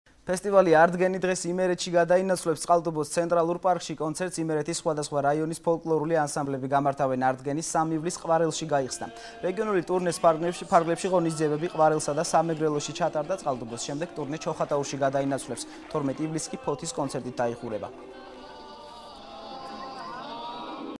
ახალი 12 | არტ-გენი იმერეთში | 08.07.12
ფესტივალი არტგენი დღეს იმერეთში გადაინაცვლებს. წყალტუბოს ცენტრალურ პარკში, კონცერტს იმერეთის სხვადასხვა რაიონის  ფოლკლორული ანსამბლები გამართავენ. არტ გენი 3 ივლისს ყვარელში გაიხსნა. რეგიონული ტურნეს ფარგლებში ღონისძიებები ყვარელსა და სამეგრელოში ჩატარდა. წყალტუბოს შემდეგ ტურნე ჩოხატაურში გადაინაცვლებს. 12 ივლისს კი ფოთის კონცერტით დაიხურება.